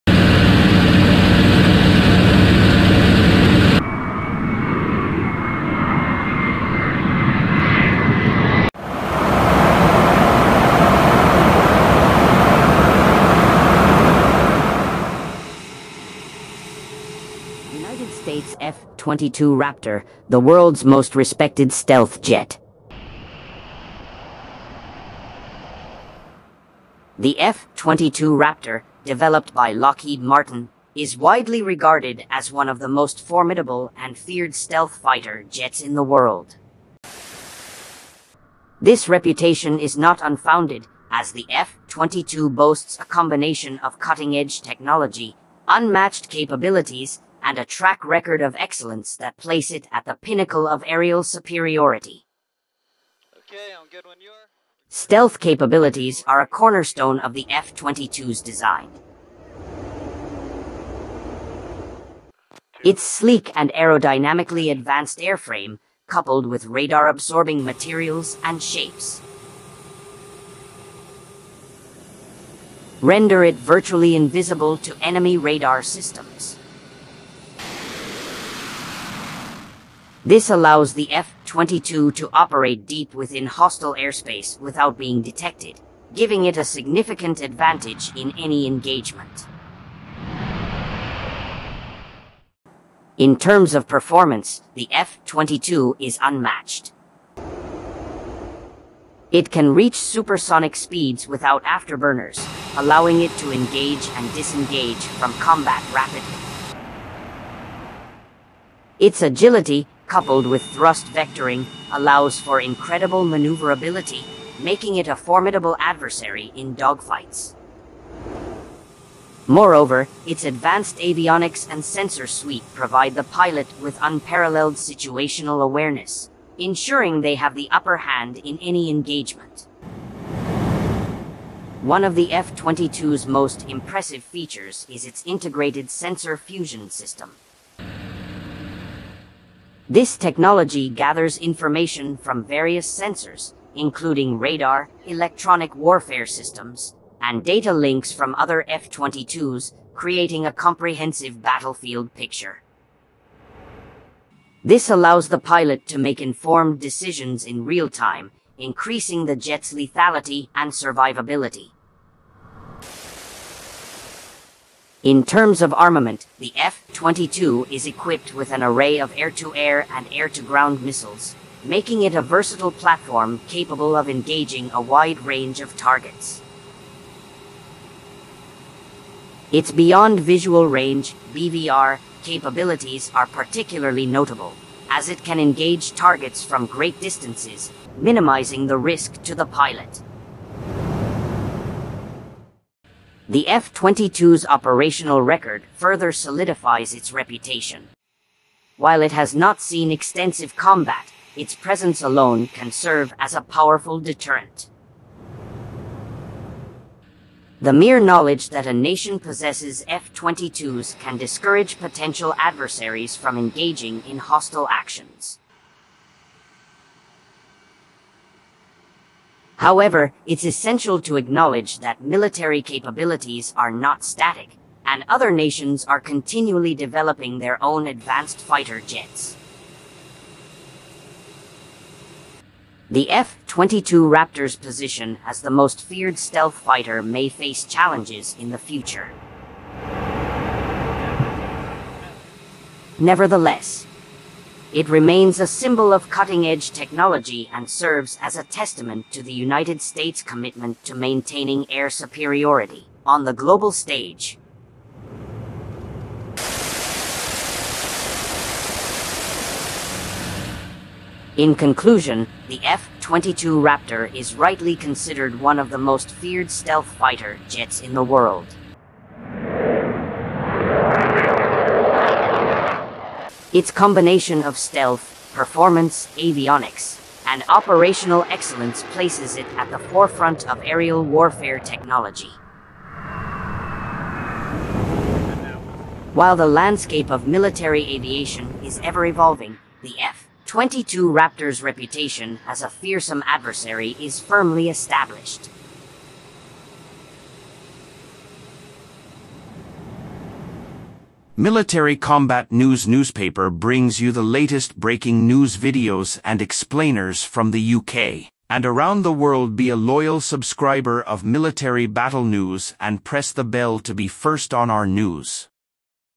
The United States F-22 Raptor Jet Is The Most Feared Stealth Fighter Jet In The World |MCN
The United States F-22 Raptor Jet Is The Most Feared Stealth Fighter Jet In The World.

If you're curious about the world's most feared stealth fighter jet then you need to watch this video! The United States F-22 Raptor Jet is the best fighter jet in the world and it's not hard to see why.

This jet is fast, strong, and completely stealthy – making it the perfect aircraft for military combat. Learn about the history of the F-22 Raptor Jet and its role in the world today in this fascinating video. Don't miss it!